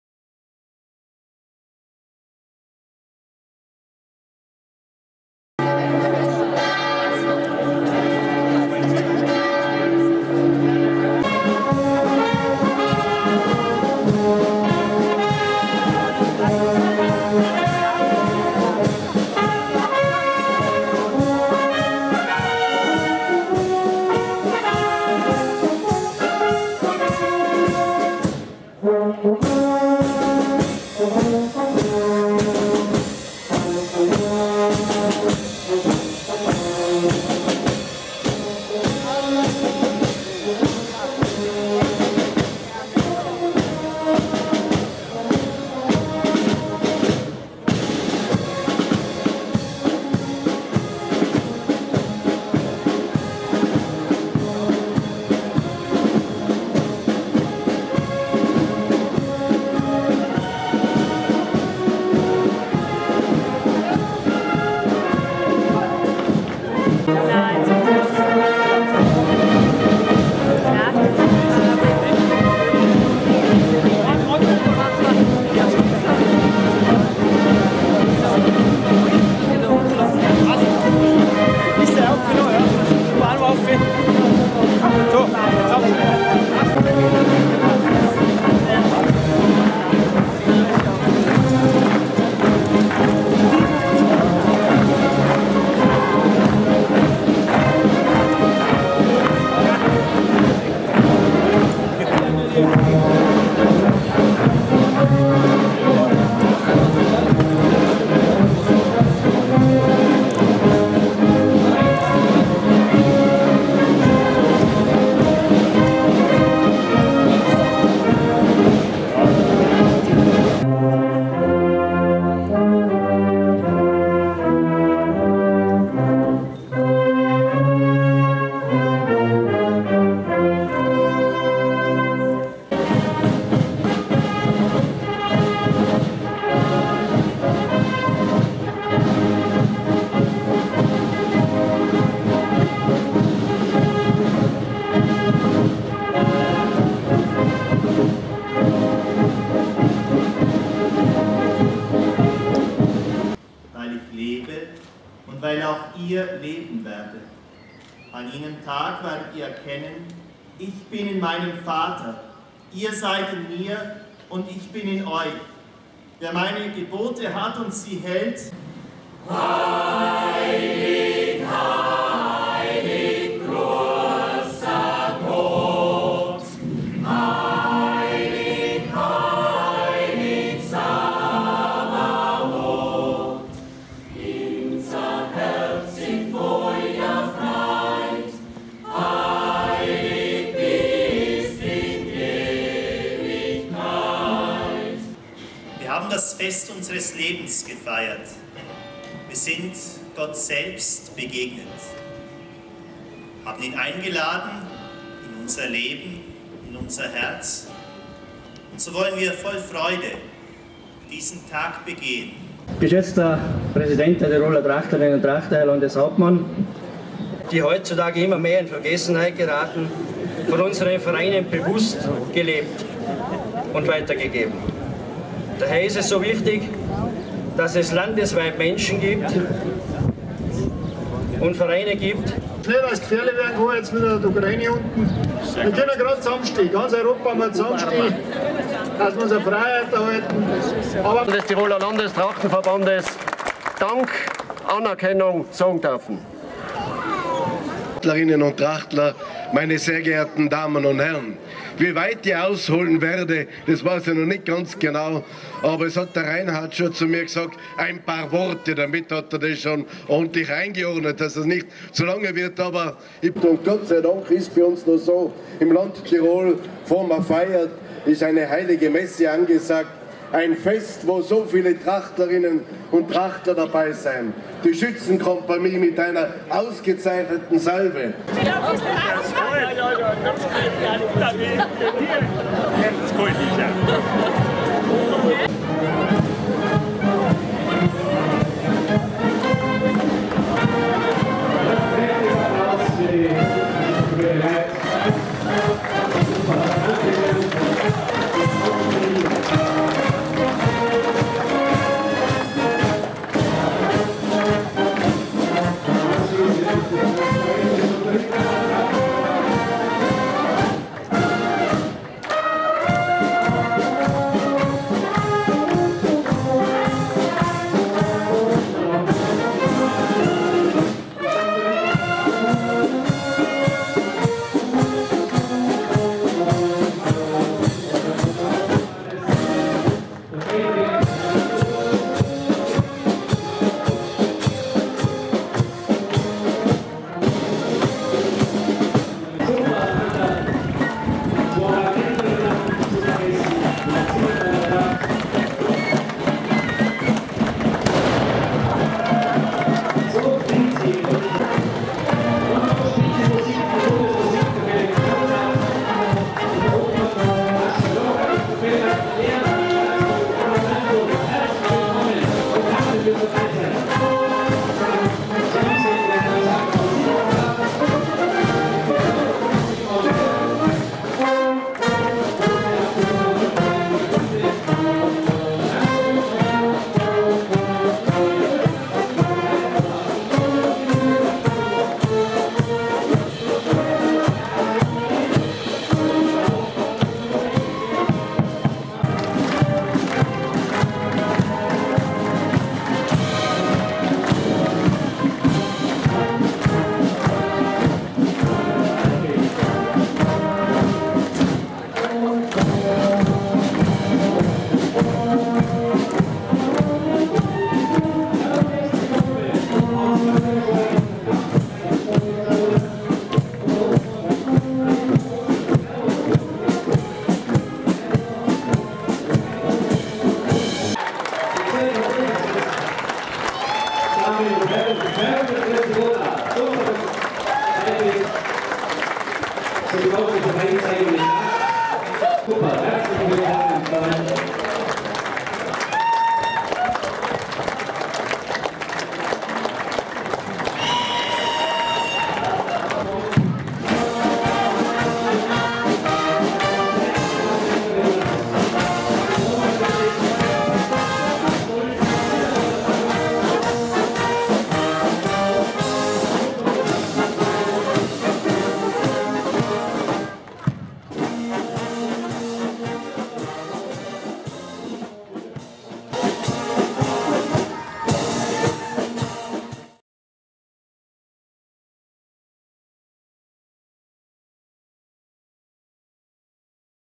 Unterinntaler Trachtenfest | ein Film von Peter Brunner (Neuhaus Peter)
Unterinntaler Trachtenfest 2014 in Reith im Alpbachtal
Das Video wurde zur Verfügung gestellt von Peter Brunner (Neuhaus Peter)